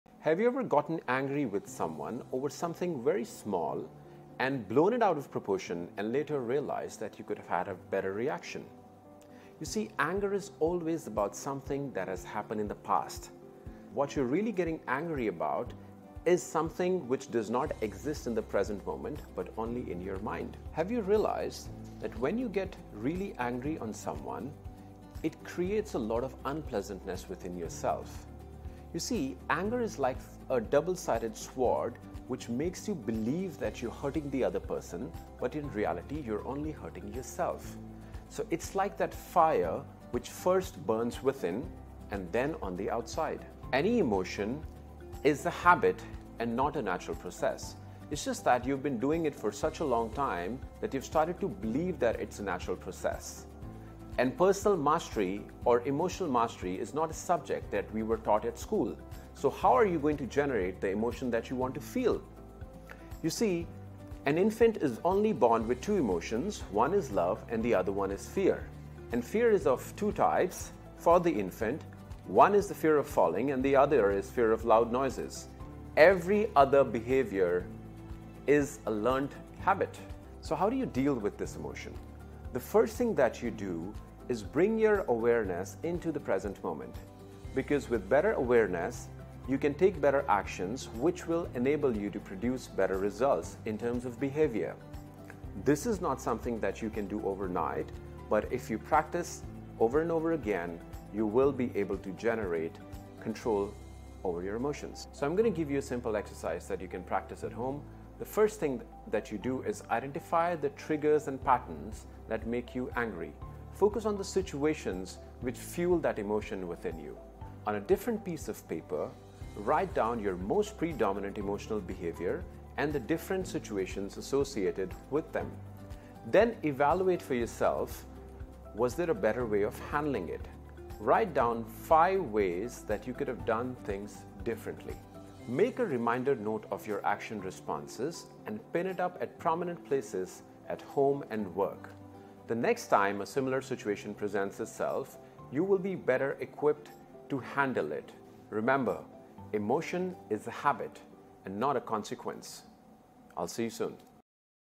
How to get angry properly
Learn to create a healthier relationship with anger